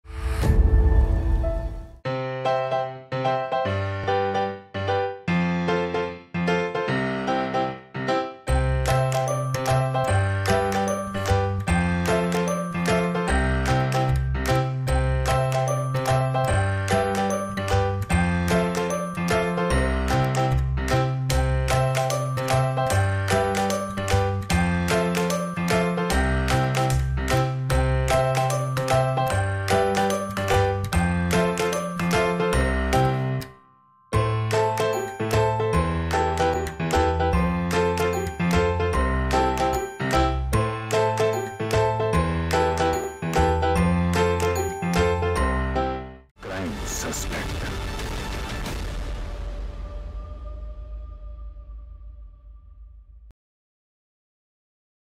MAKING
First day shoot, everyone on the sets are very excited for our new venture. We are getting full support from the director and artist. We are ready to do something different by stepping out of our zone.